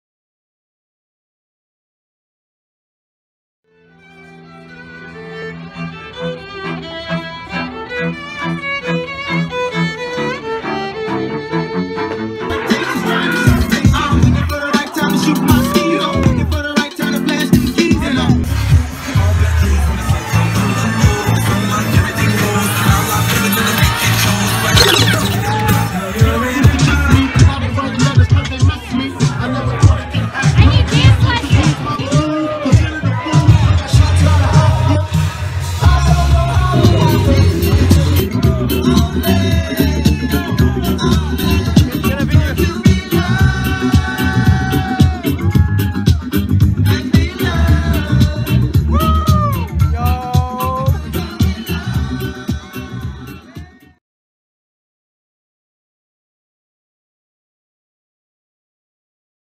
NY Beats DJs Gramercy Park Hotel wedding in New York, NY DJ Trip-Set NYC DJ
NY wedding DJ in New York City. NY Beats DJs present DJ Trip-Set at Gramercy Park Hotel 7th Ave Strings (trio or quartet) performed the wedding ceremony and cocktail hour also at the GCP.